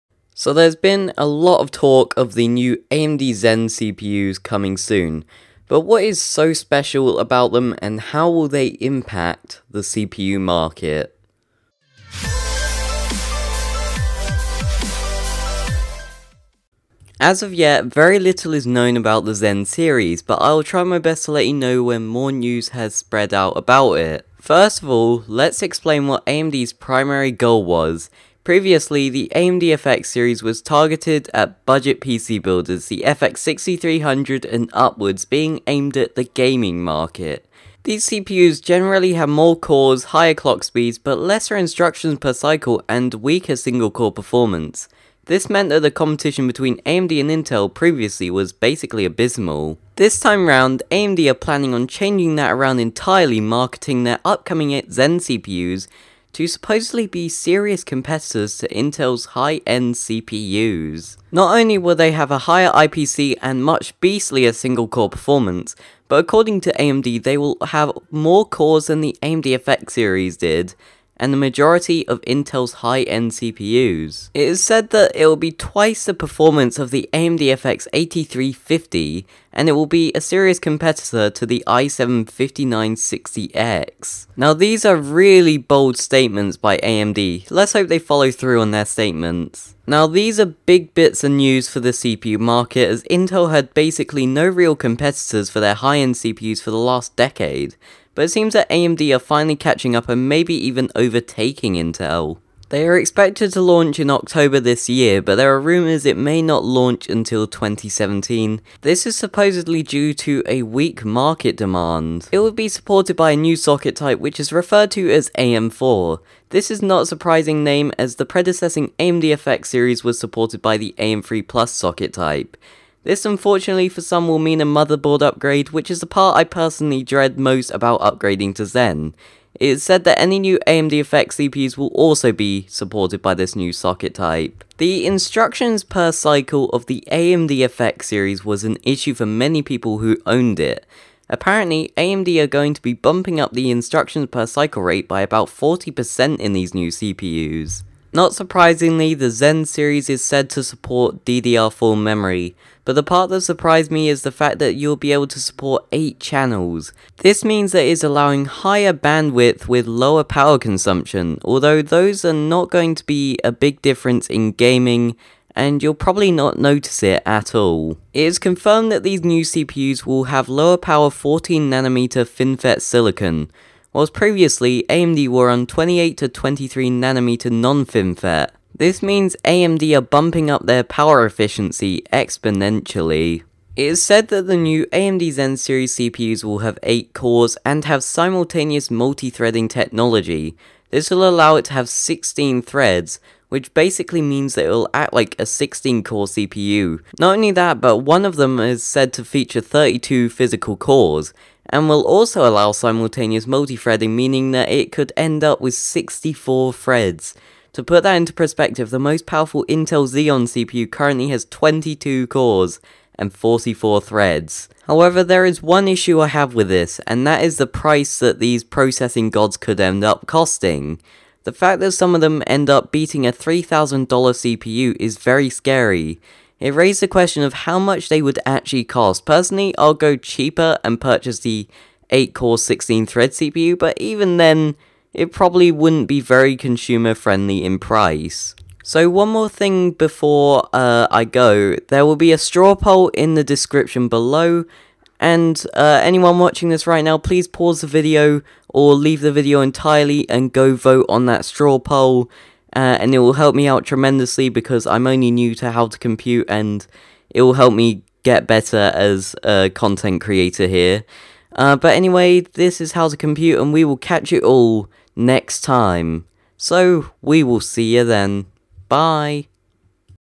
AMD Zen - Intel Will Finally Have Competition!
Today I cover the topic of the upcoming AMD Zen CPU expected to launch October